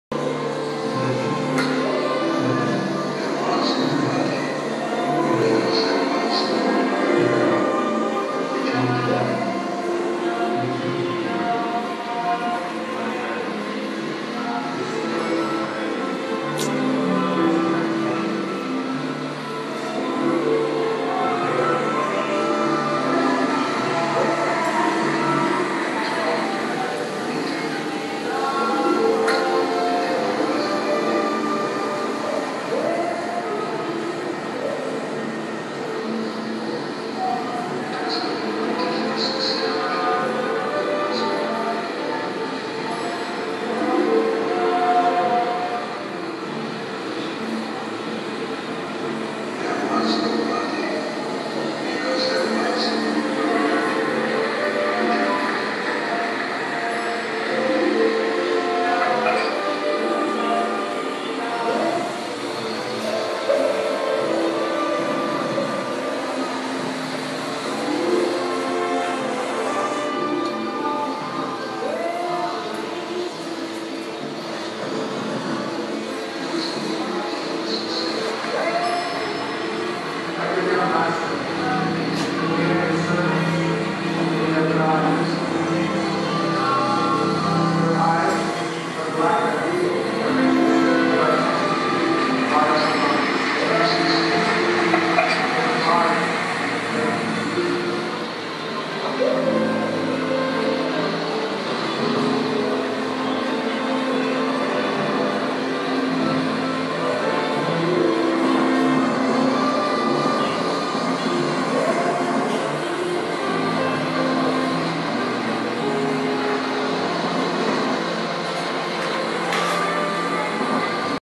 abc - art berlin contemporary 2010 @ Galerie Olaf Stüber
BitteBitteJaJa ( Ulu Braun & Roland Rauschmeier) with 3 videos from series "Cadvare Exquis Vivant" at Galerie Olaf Stüber